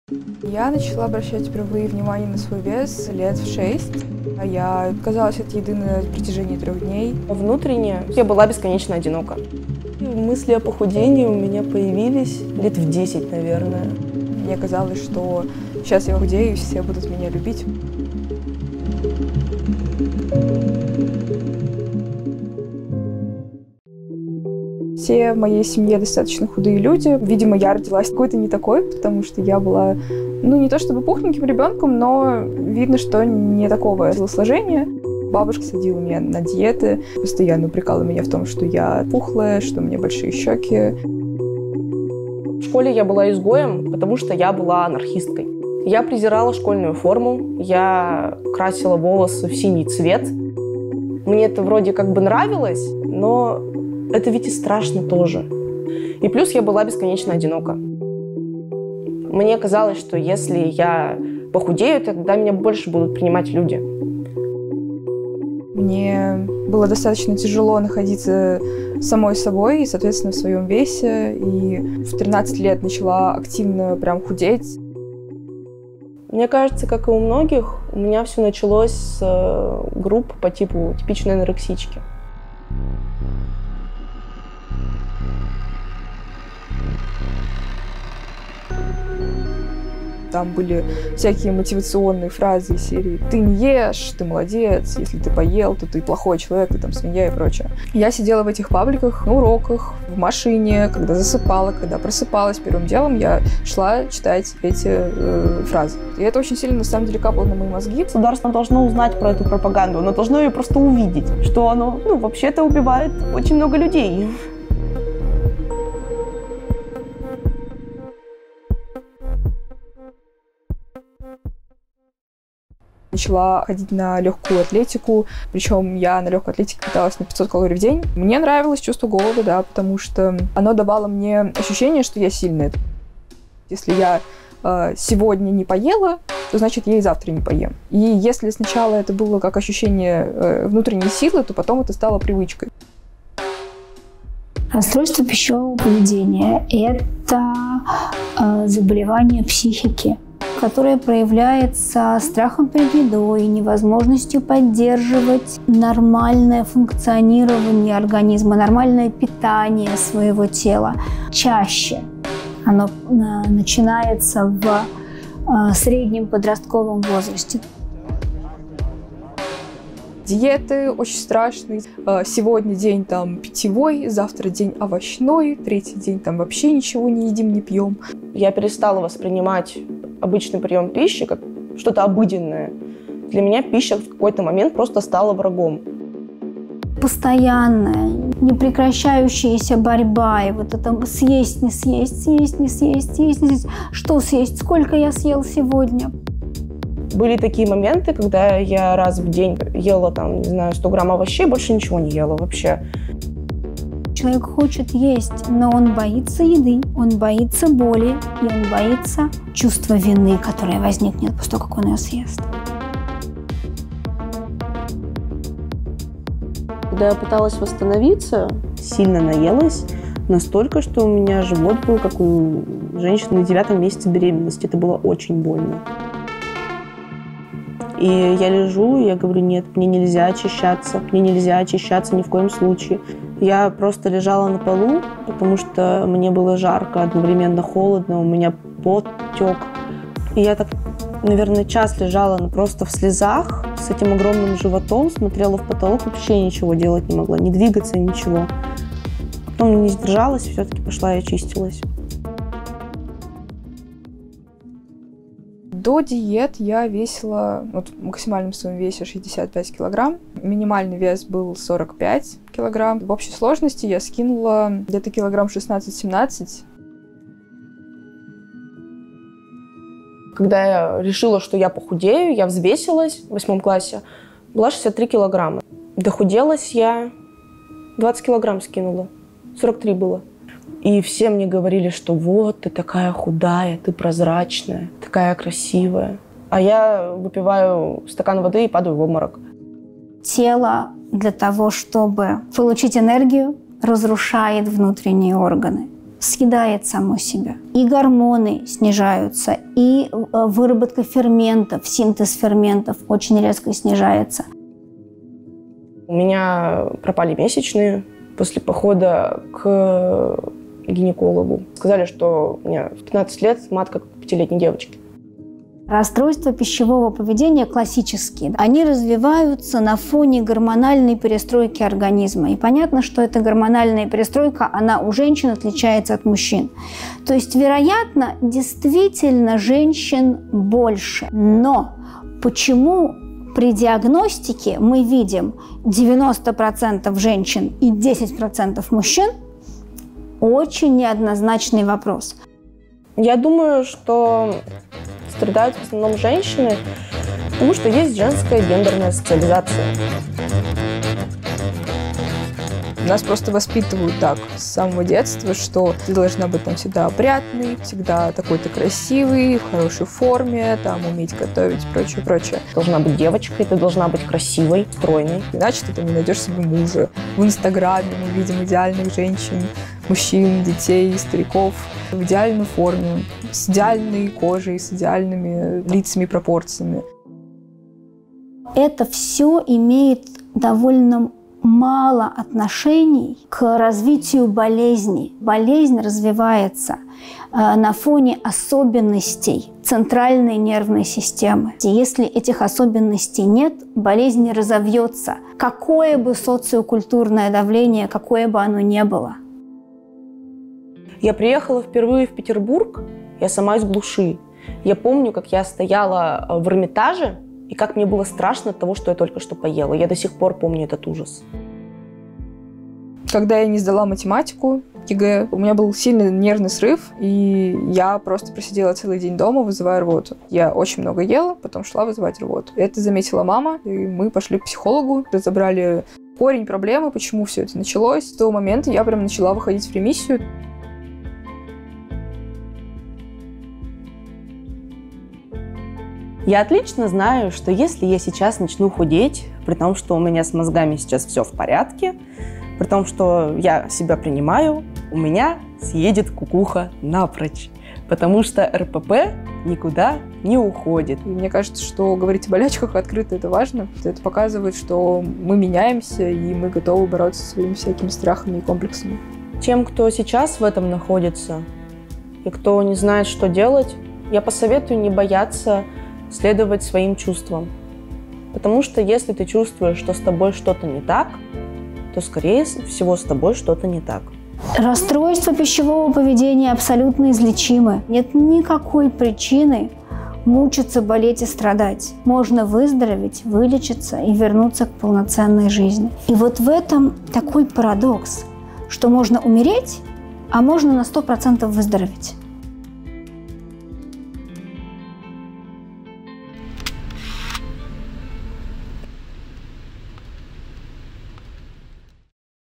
"Мне нравилось чувство голода"
В самый острый момент болезни у Светы выпадали волосы. Размер ее внутренних органов уменьшился — они могли бы показаться детскими. Тело, как говорят врачи, съедало само себя. «Расстройство пищевого поведения — это болото, из которого очень сложно выбраться и [в которое] очень легко попасть», — говорит она.
РПП может случиться с каждым. Чаще всего оно диагностируется у девочек подросткового возраста.
О том, что вынуждает человека отказаться от еды — в материале «Таких дел».